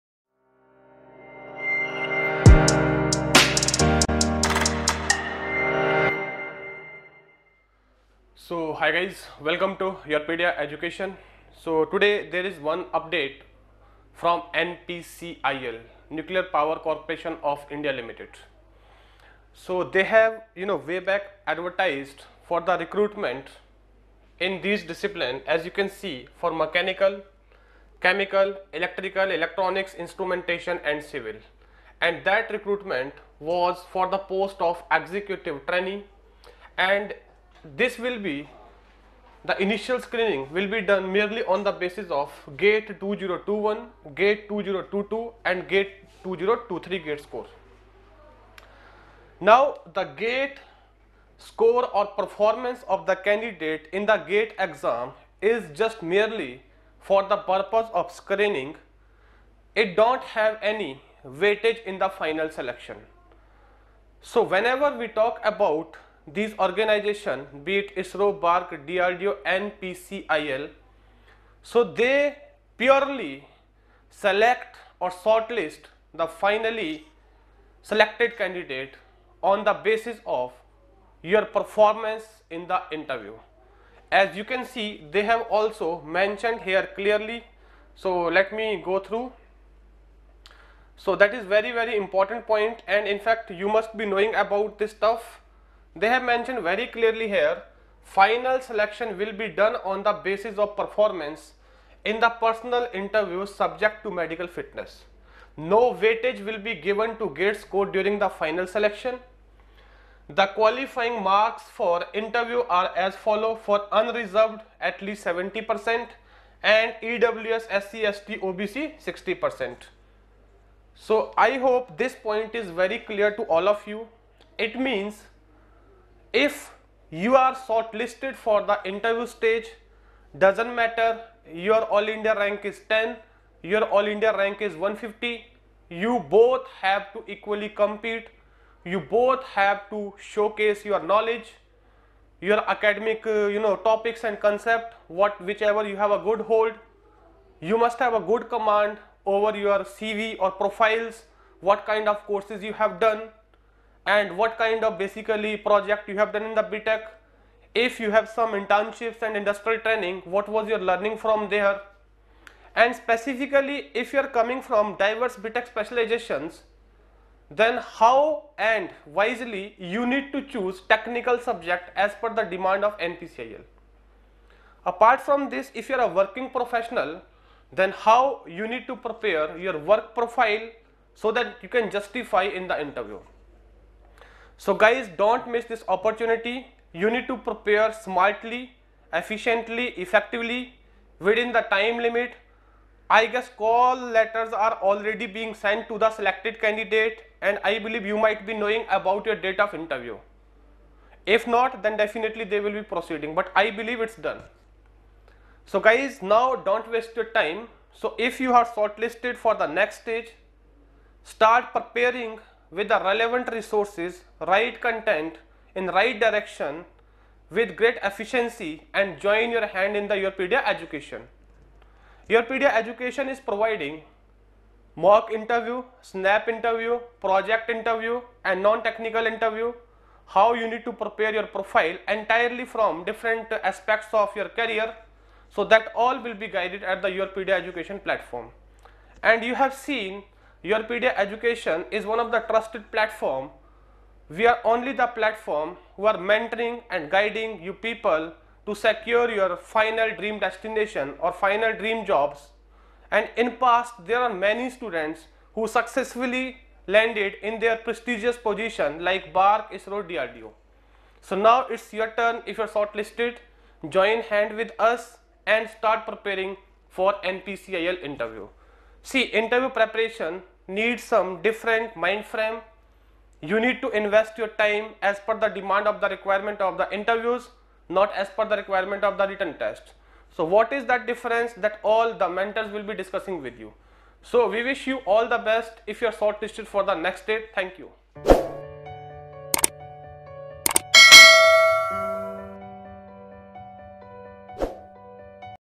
NPCIL final selection purely on basis of Interview | Shortlisting out | Preparation & guidance
To directly join the module (Package 1)

The Nuclear Power Corporation of India Limited (NPCIL) is an Indian public sector undertaking based in Mumbai, Maharashtra. It is wholly owned by the Government of India and is responsible for the generation of nuclear power for electricity. NPCIL is administered by the Department of Atomic Energy (DAE).

NPCIL was created in September 1987 under the Companies Act 1956, "with the objective of undertaking the design, construction, operation and maintenance of the atomic power stations for generation of electricity in pursuance of the schemes and programmes of the Government of India under the provision of the Atomic Energy Act 1962." All nuclear power plants operated by the company are certified for ISO-14001 (Environment Management System).